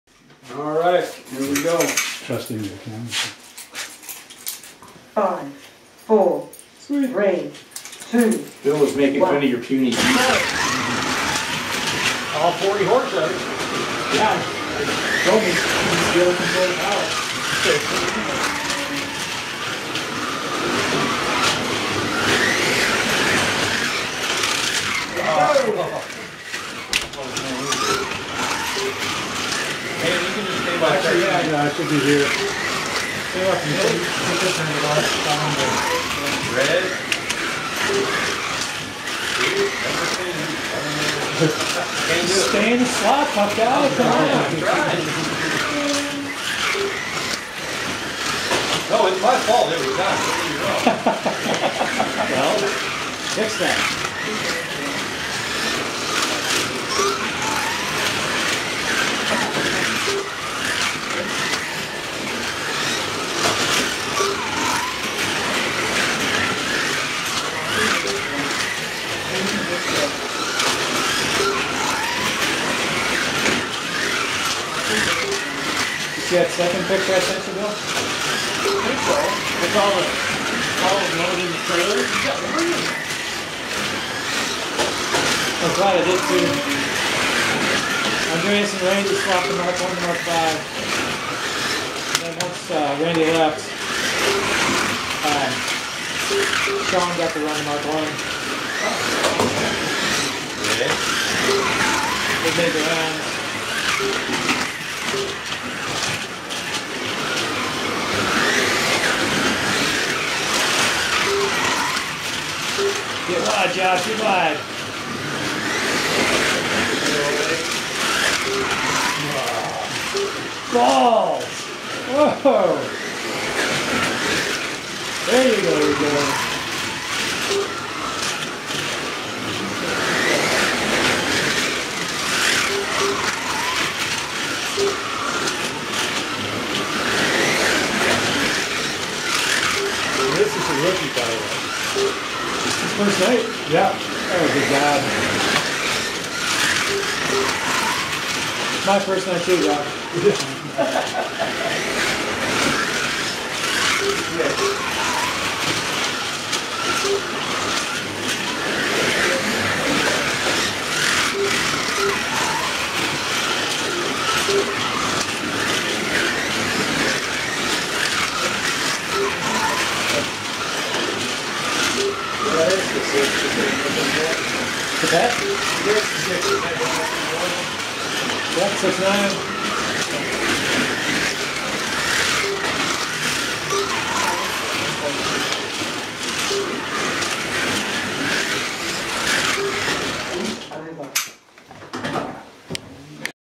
1/24 BRM Group C racing at Rippin' Ridin' Raceway on June 13, 2019. Part 1